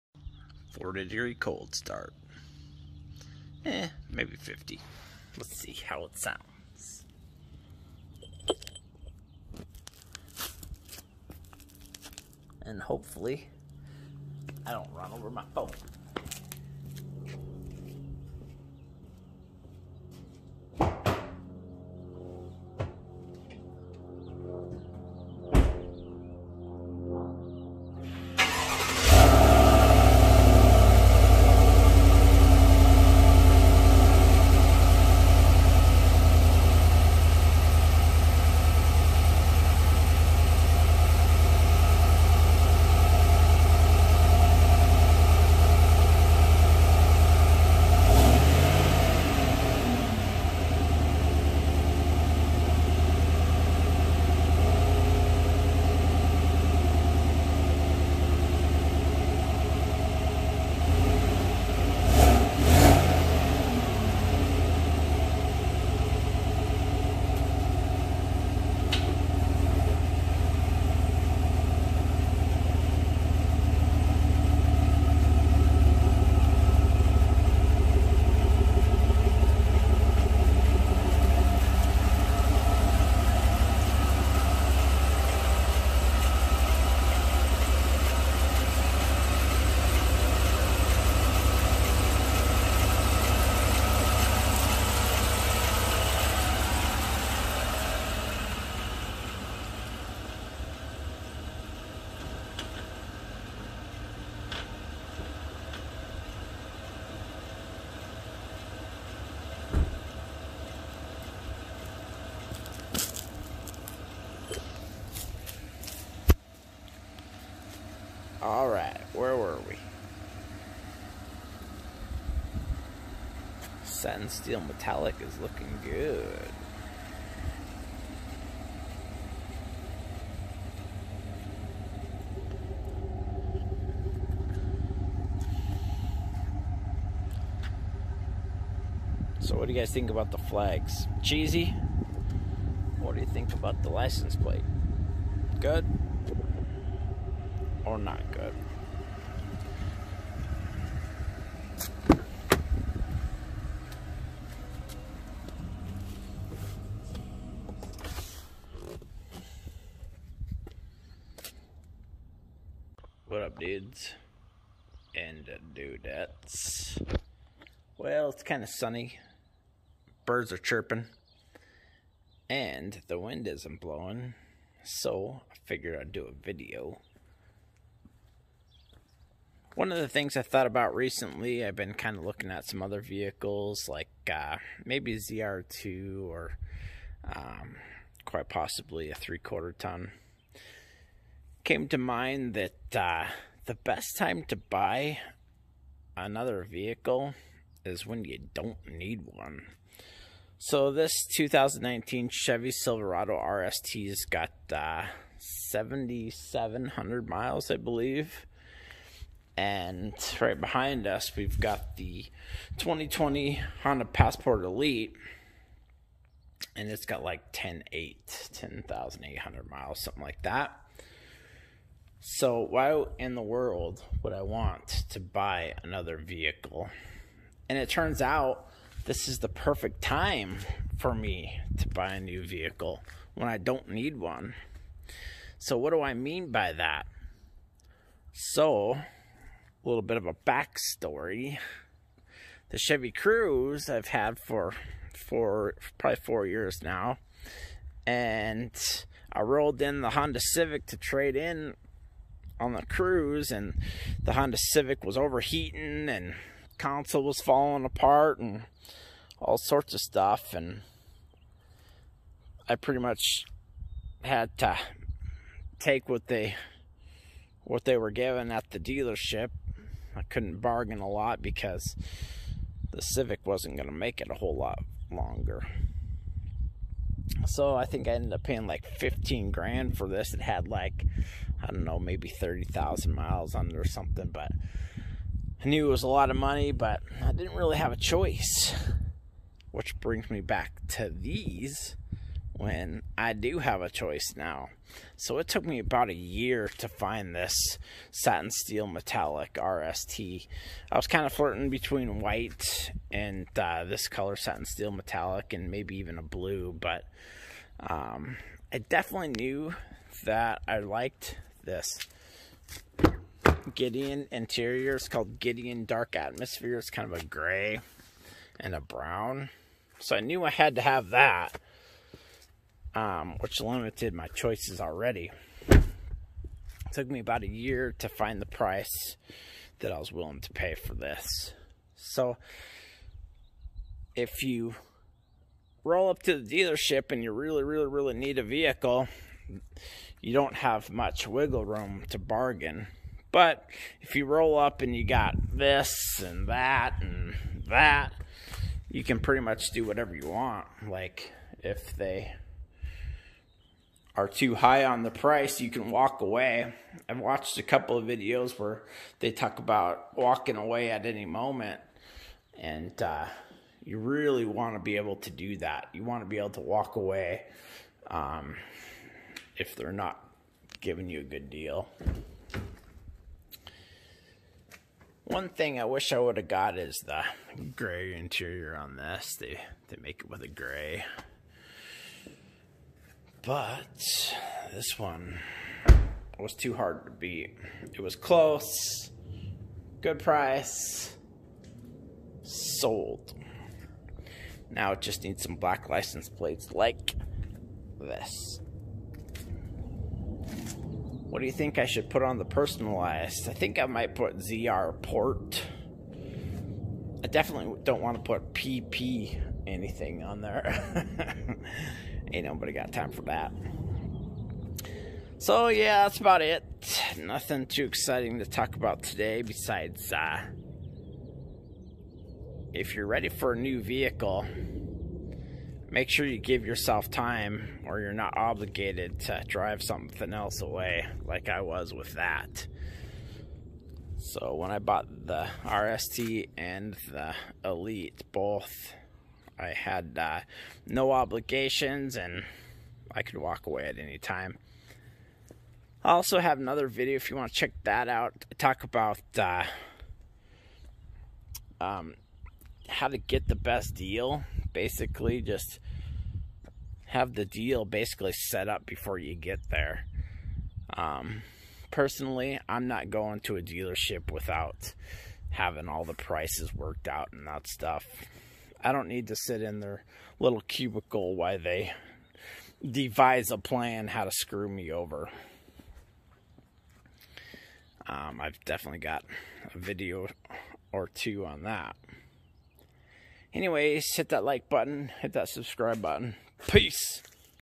Best time to buy a car? Featuring 2020 Honda passport Elite/Silverado RST
I talk about why the best time to buy a car is actually when you don't need one